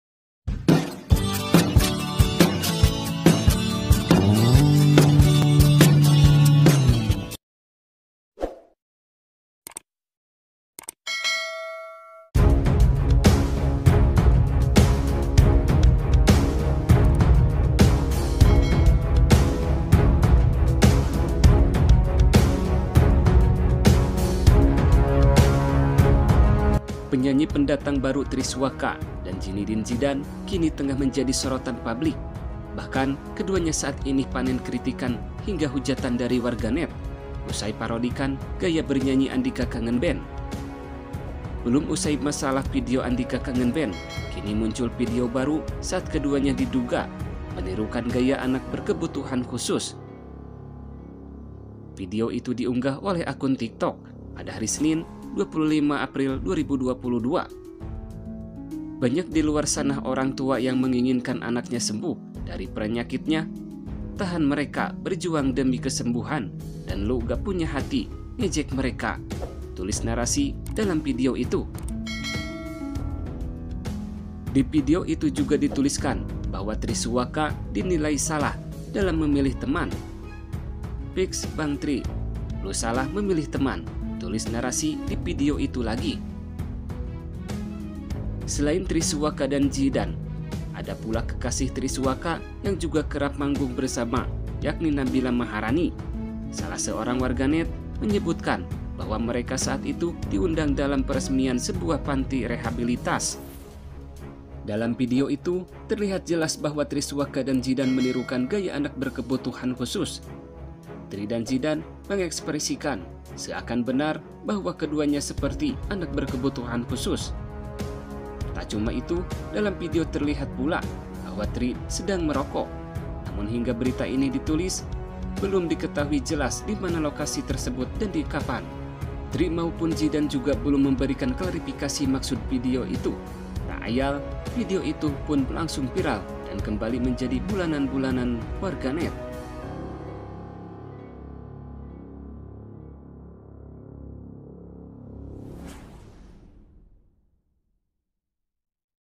GER4M!Netizen Menghujat Kritikan PEDAS Usai Zidan & Tri Suaka Tirukan Gaya Orang Berkebutuhan khusus
SAEPUL R channel. Penyanyi pendatang baru Tri Suaka dan Zinidin Zidan kini tengah menjadi sorotan publik. Bahkan, keduanya saat ini panen kritikan hingga hujatan dari warganet usai parodikan gaya bernyanyi Andika Kangen Band.

Belum usai permasalahan video Andika Kangen Band, kini muncul video baru saat keduanya diduga menirukan gaya anak berkebutuhan khusus.

Video itu diunggah oleh akun TikTok @zie_fm, Senin (25/4/2022). "Banyak di luar sana orang tua yang menginginkan anaknya sembuh dari penyakitnya. Tahan mereka berjuang demi kesembuhannya dan lu ga punya hati ngejek mereka," tulis narasi dalam video itu.